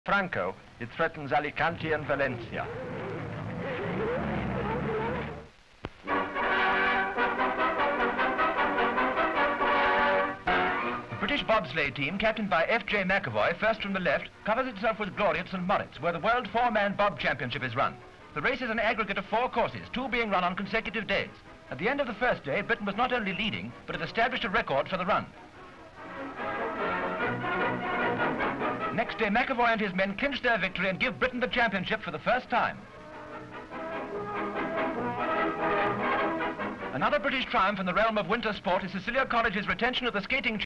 Bobsleighing
Shot of the British team including MacAvoy [sic]. Shot of the team breaking the record.